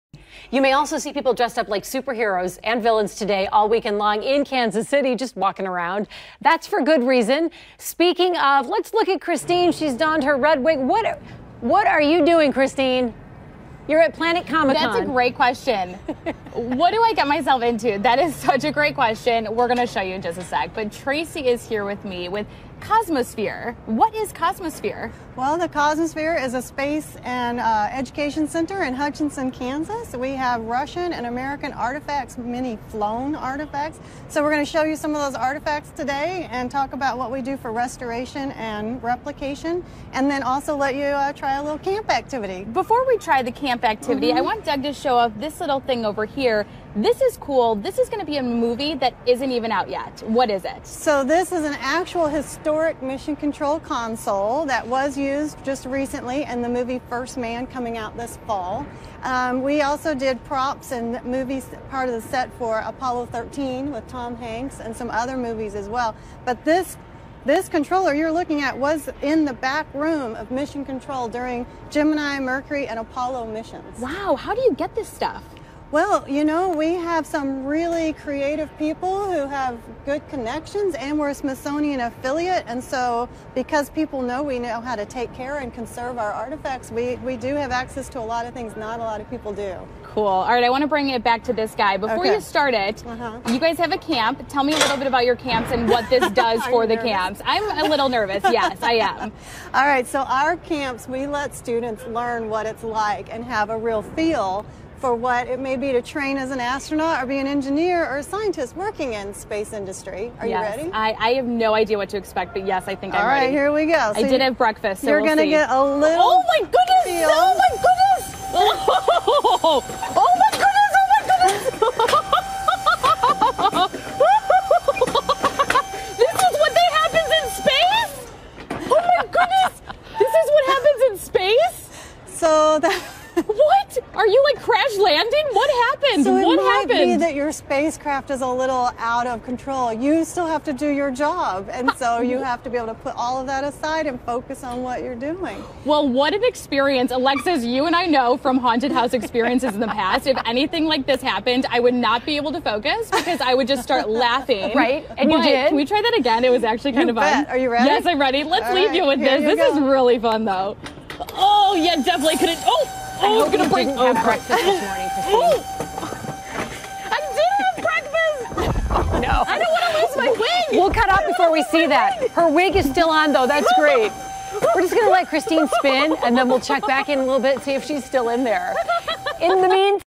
Watch Christine's world get turned upside down at Planet Comicon Kansas City
Cosmosphere COO Tracey Tomme provides Christine with an out-of-this-world experience taking place at Planet Comicon Kansas City.